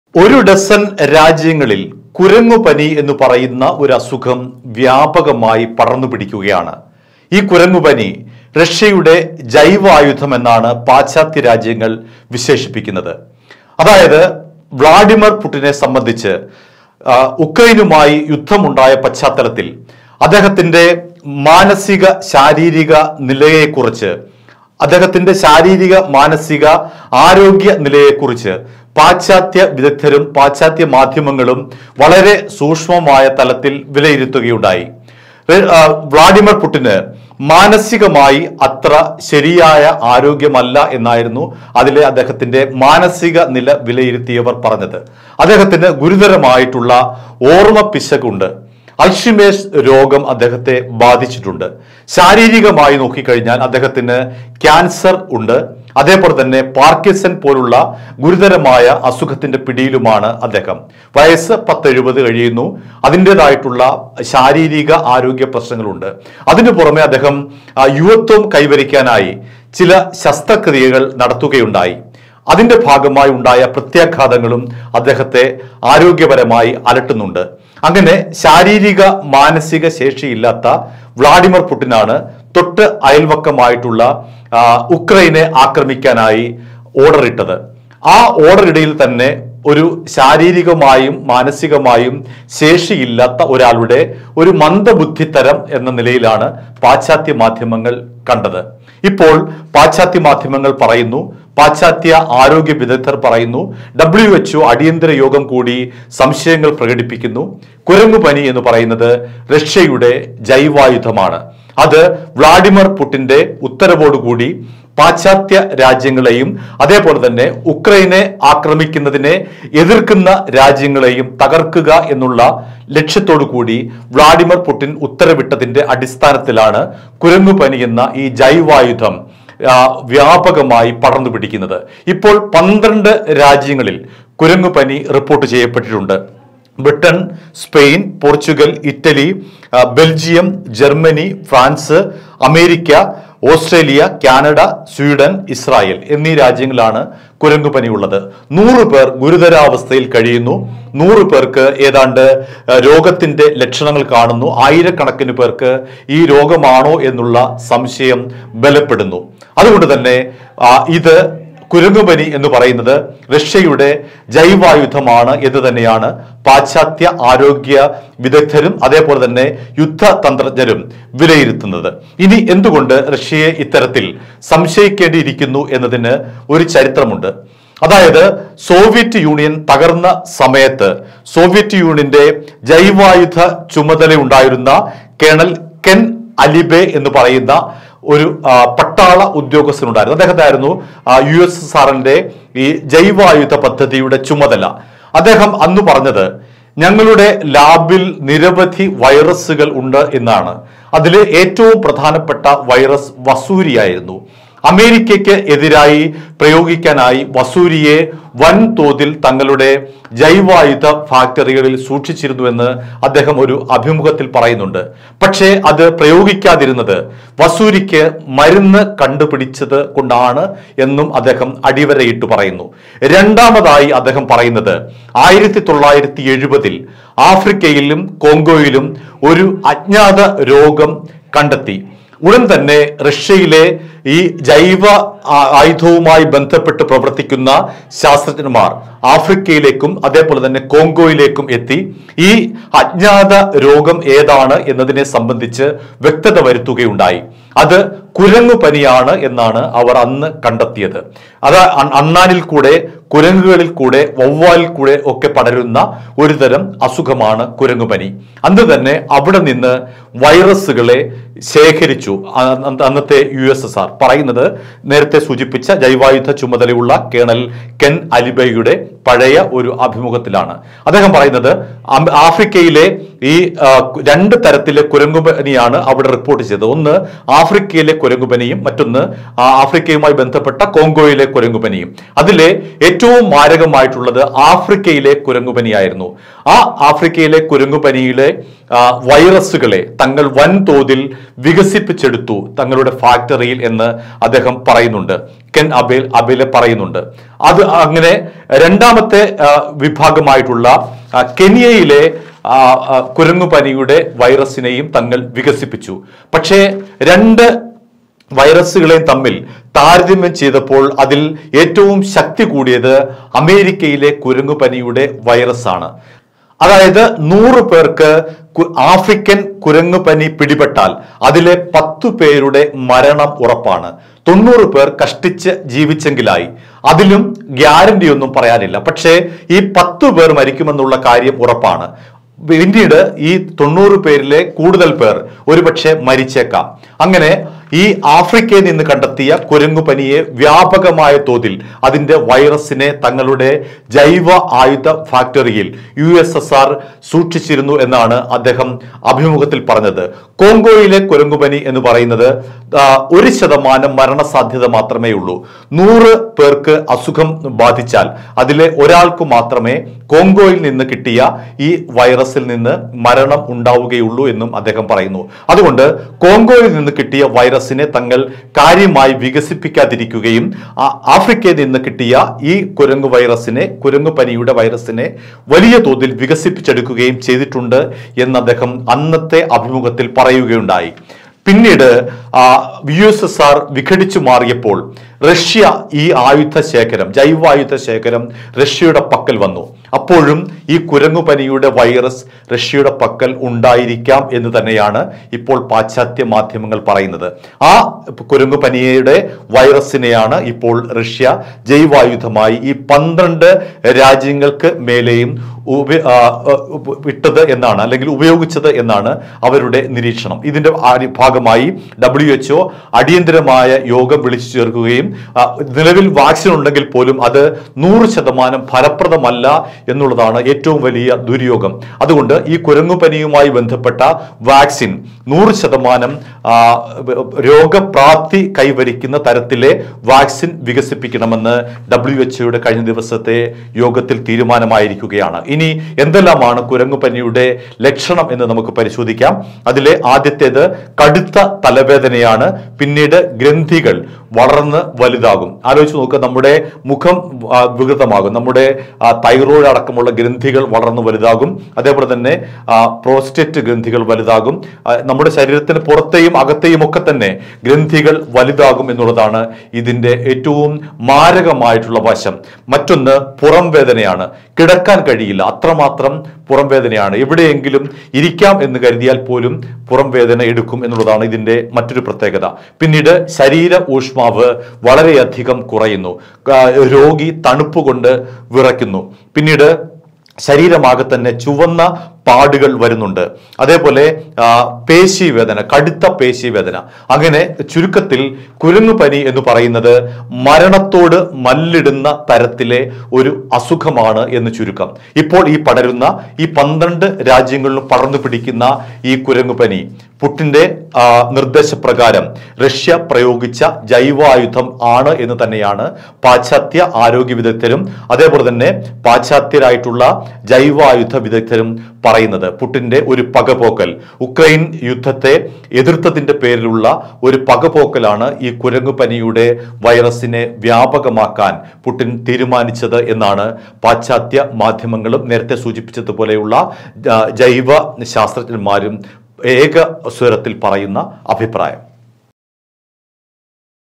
ഞെട്ടിവിറച്ച് പാശ്ചാത്യരാജ്യങ്ങള്‍ I Russia
അമേരിക്ക അടക്കമുള്ള പന്ത്രണ്ടോളം രാജ്യങ്ങളില്‍ പടരുന്ന കുരങ്ങുപനി റഷ്യയുടെ ജൈവായുധമാണെന്ന് വിദഗ്ധര്‍. ആരോപണത്തിന്റെ പശ്ചാത്തലത്തില്‍ ലോകാരോഗ്യസംഘടന അടിയന്തിര യോഗം ചേര്‍ന്നു